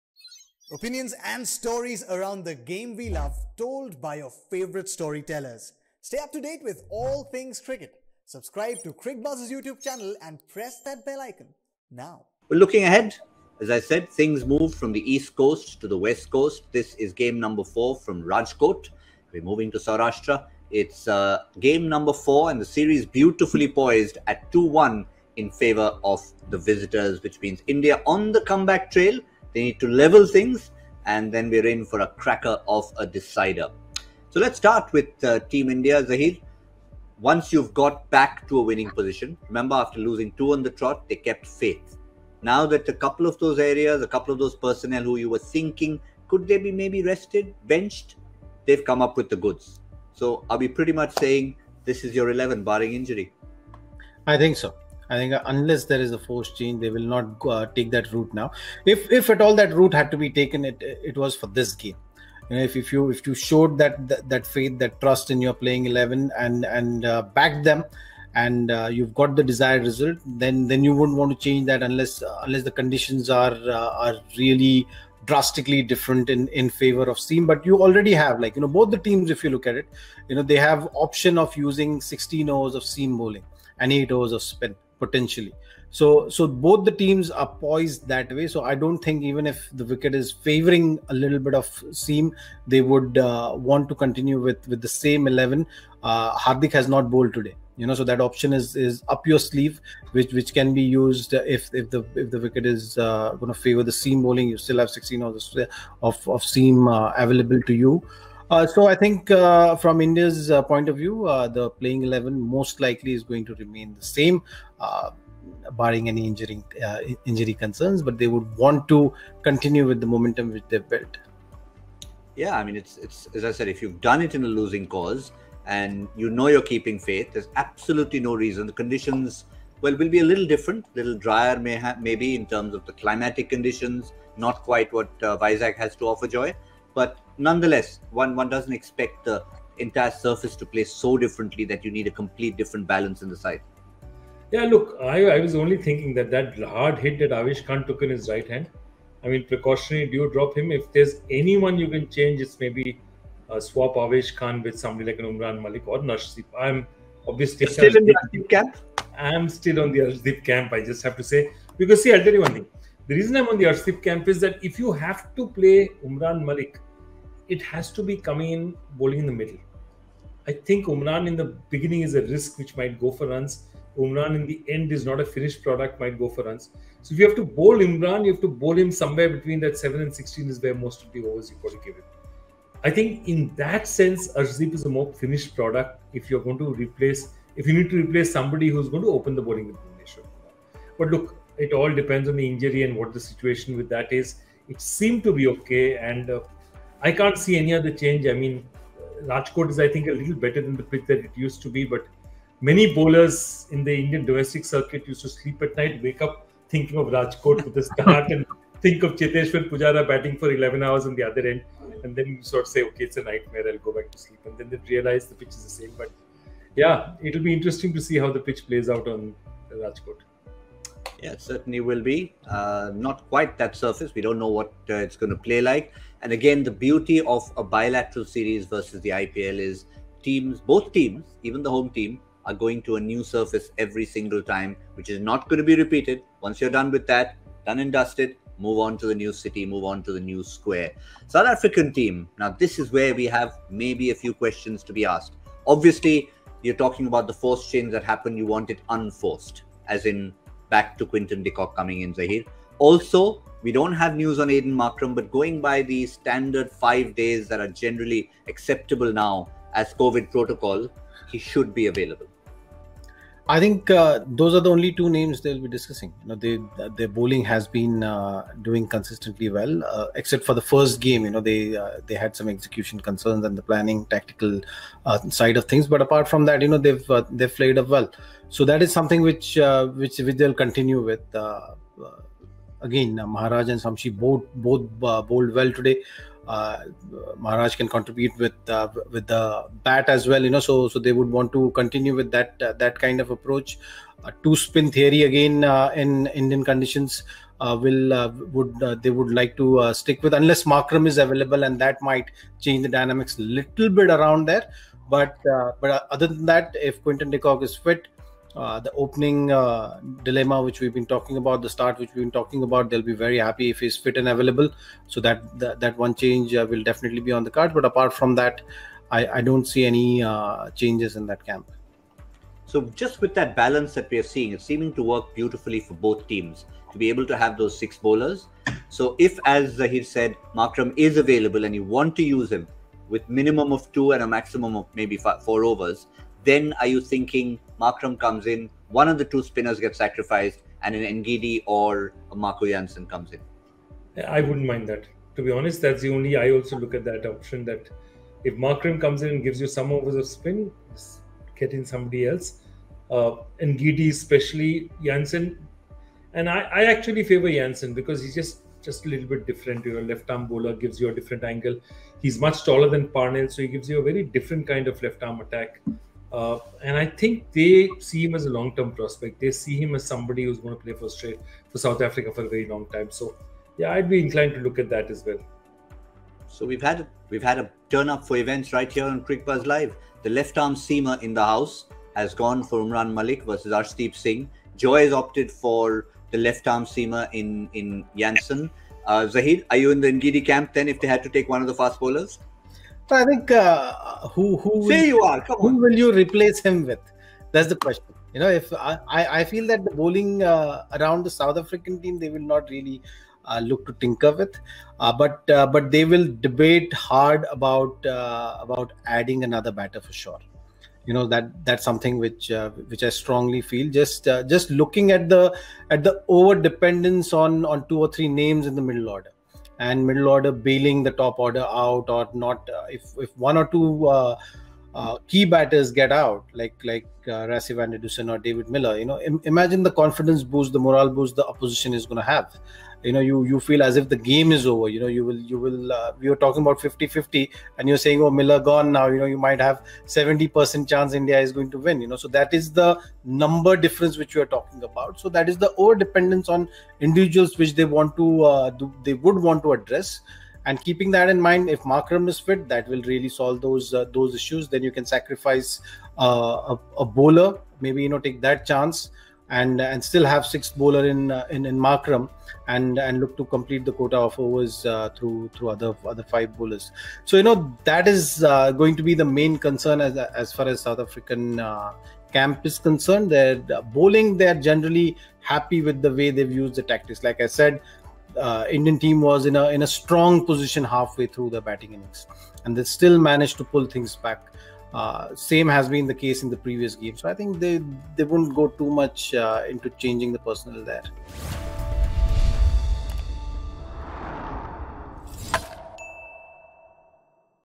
INDvSA | India v South Africa, 4th T20I: Preview
Can India draw level at 2-2 with a win in Rajkot or will SA bounce back to clinch the series? Zaheer Khan & Joy Bhattacharjya preview the 4th T20I, on Cricbuzz Live